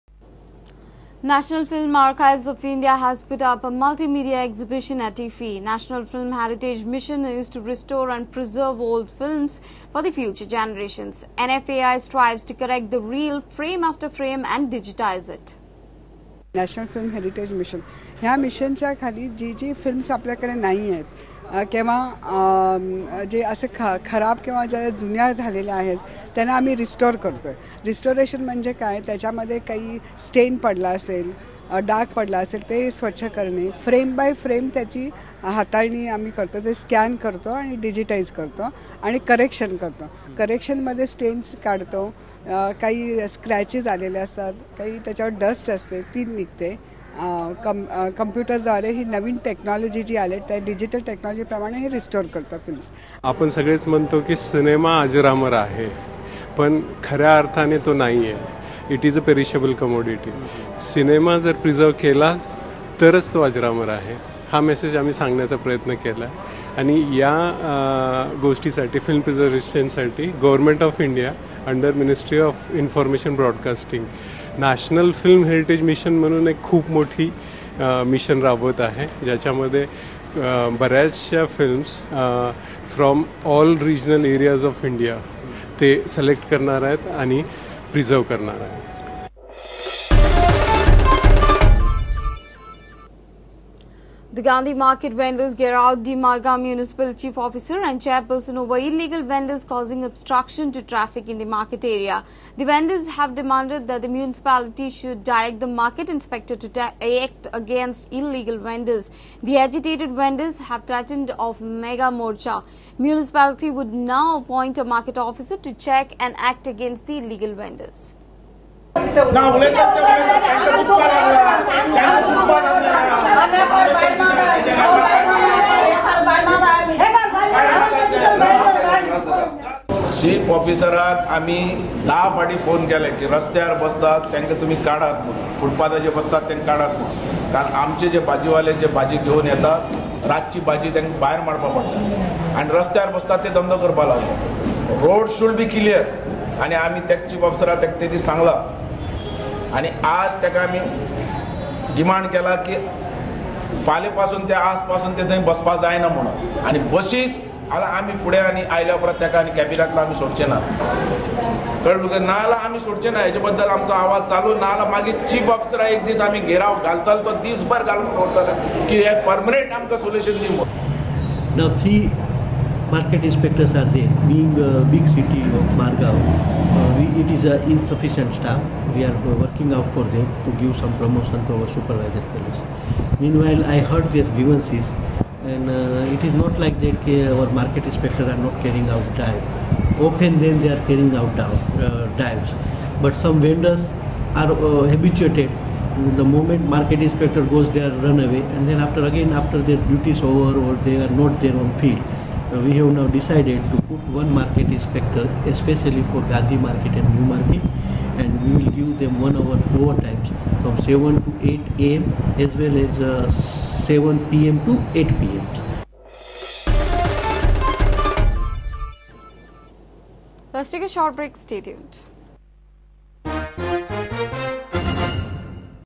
Prudent Media English Prime News 26 Nov 15 Part 2
National Film Archives of India has put up a multimedia exhibition at  IFFI.